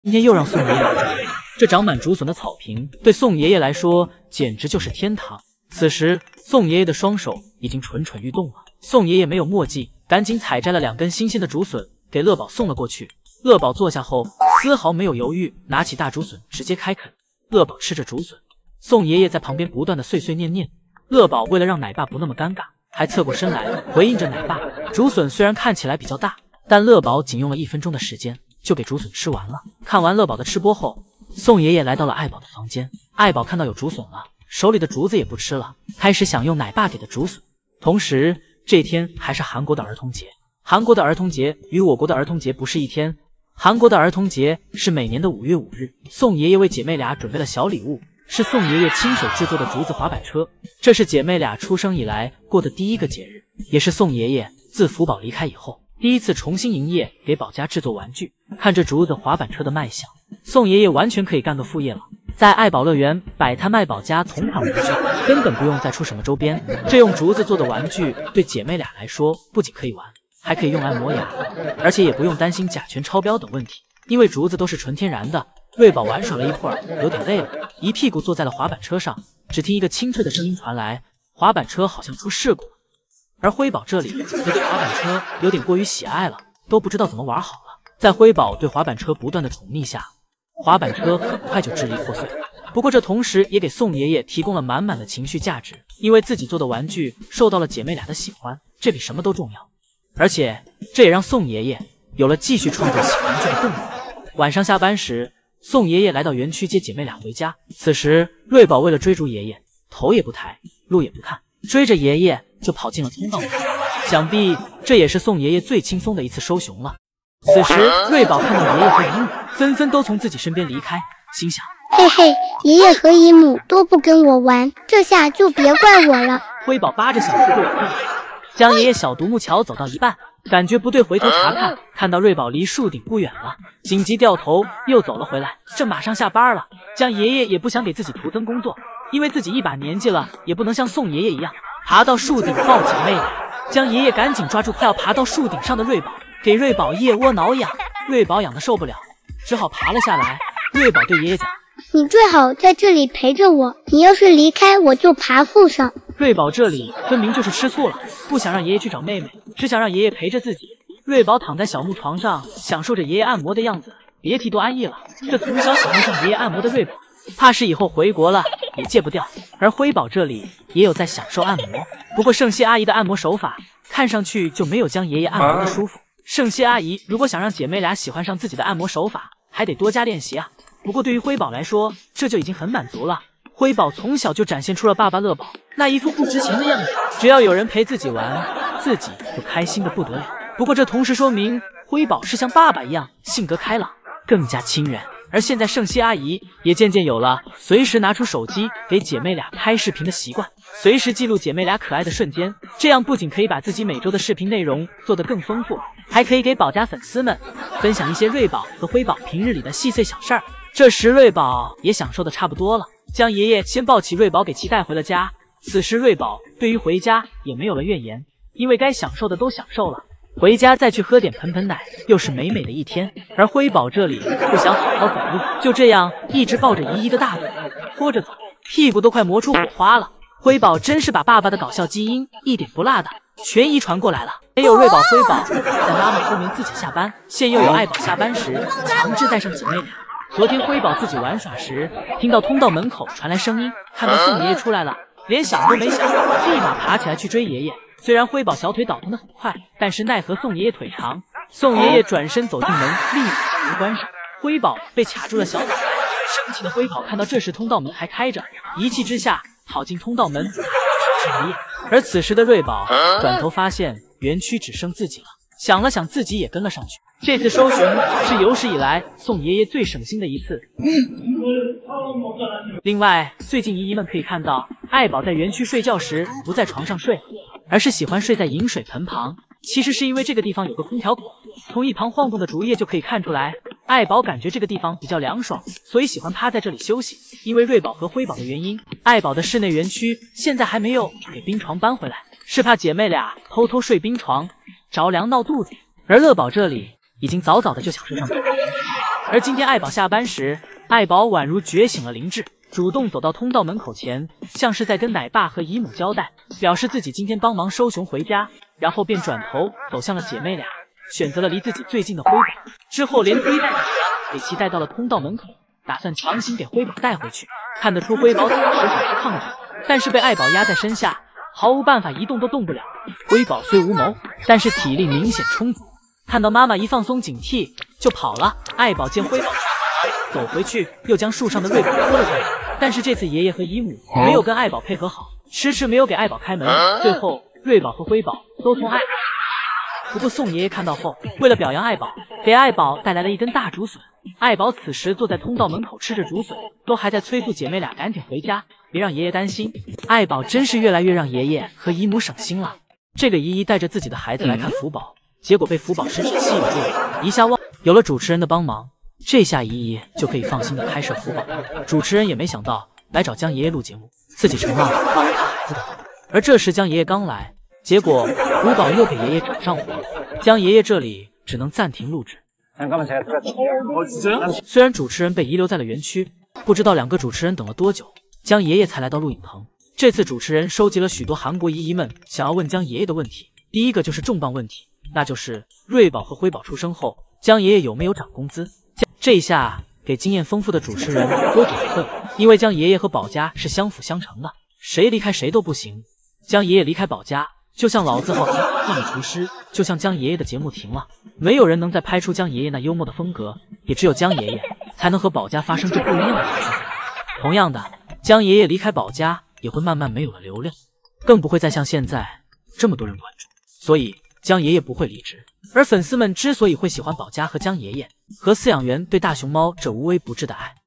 今天宋爺爺發現一大片竹筍，帶回去樂寶給樂寶直接開啟吃播，這吃的也太香了！ 【有趣的喵哇】
今天宋爺爺發現一大片竹筍，帶回去樂寶給樂寶直接開啟吃播，這吃的也太香了！
哈嘍，這裡是有趣的喵哇~
陪你趣談科普
更新熊貓日常生活
搞笑的畫面總能打動你